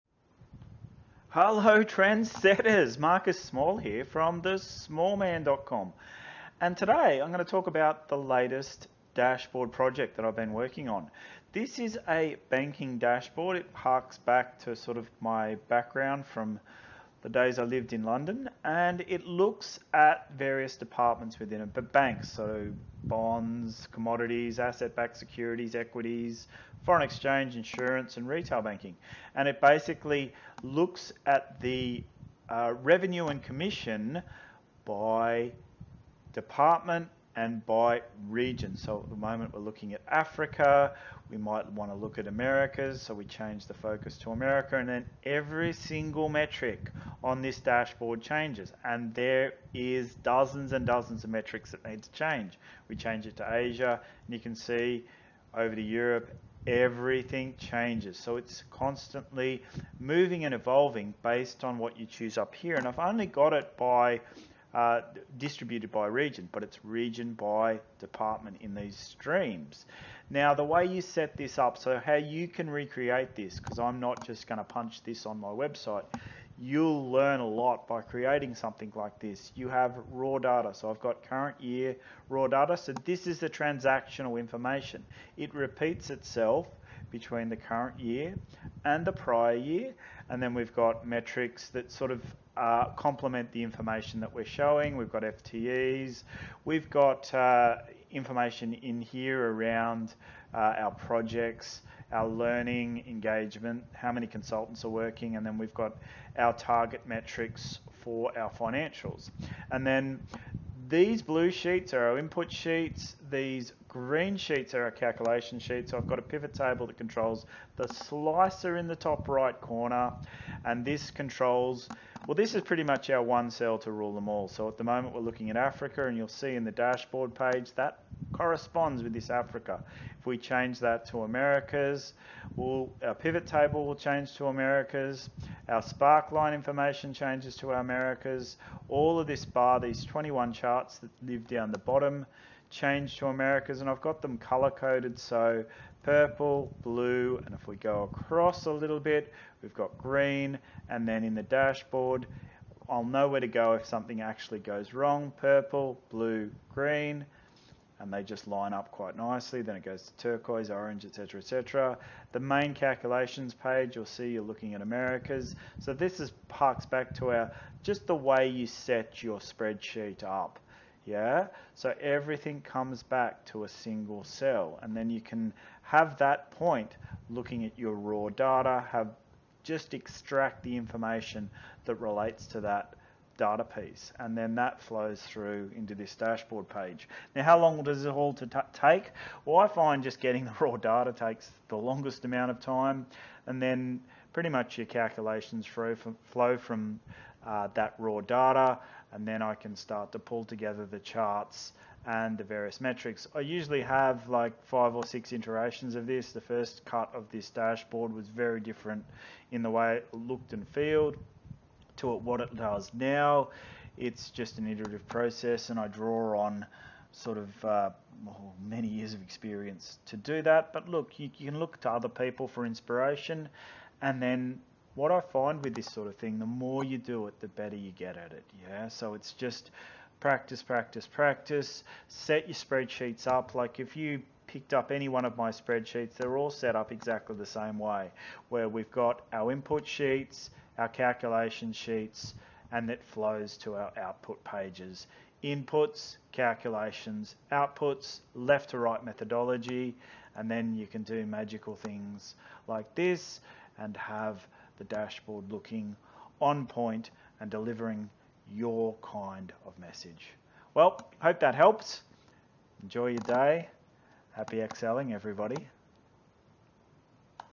Excel Dashboard Design
Latest dashboard project, showing the setup of a project from start to finish.